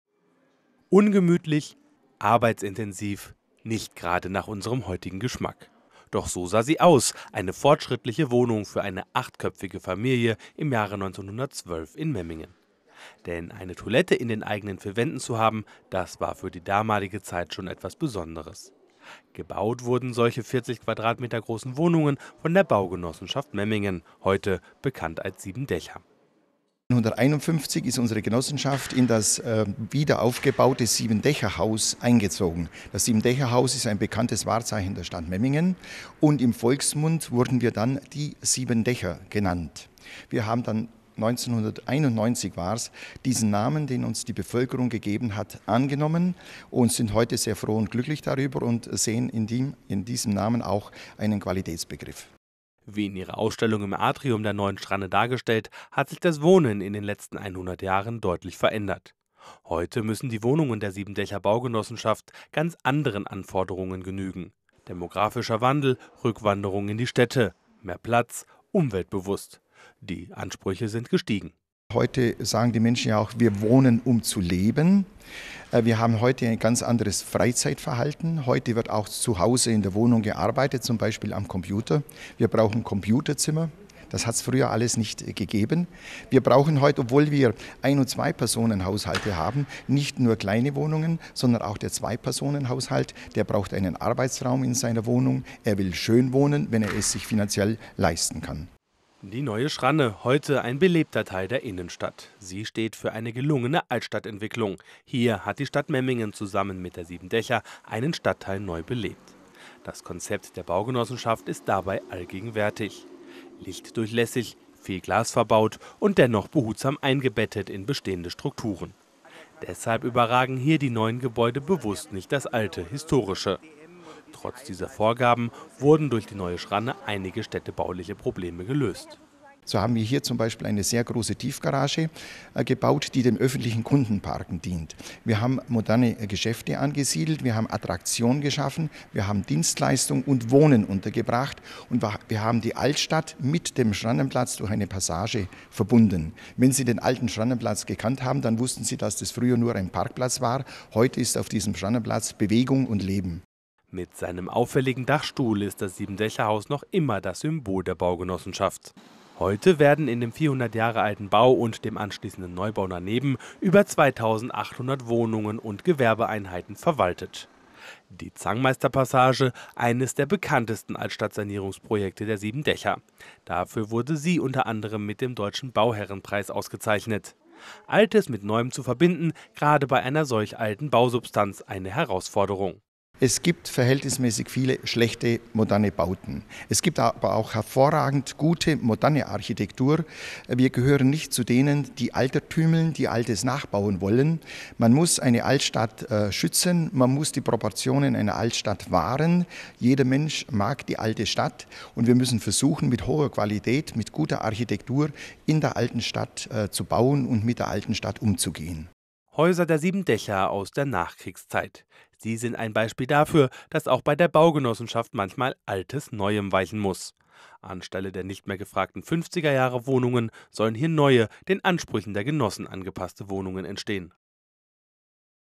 100 Jahre Tradition: Siebendächer Baugenossenschaft verbindet Altes mit Neuem
Seit heute kann im Memminger Siebendächerhaus wieder durchgeatmet werden. Nach dem Chemieunfall in einer Apotheke war das Memminger Wahrzeichen für einige Wochen gesperrt. Grund: Giftige Dämpfe machten ein Arbeiten unmöglich. Nun aber sind die Richtwerte - laut einem Sachverständigen - wieder im grünen Bereich. Und das heißt auch für die Mitarbeiter der Siebendächer-Baugenossenschaft - deren Arbeit wir ihnen heute einmal vorstellen wollen- dass es wieder normal weitergehen kann. Die Baugenossenschaft Siebendächer baut und saniert für ihre Mitglieder - und das ist manchmal auch ein Spagat zwischen dem Erhalt historischer Bausubstanz und modernen Wohnwünschen.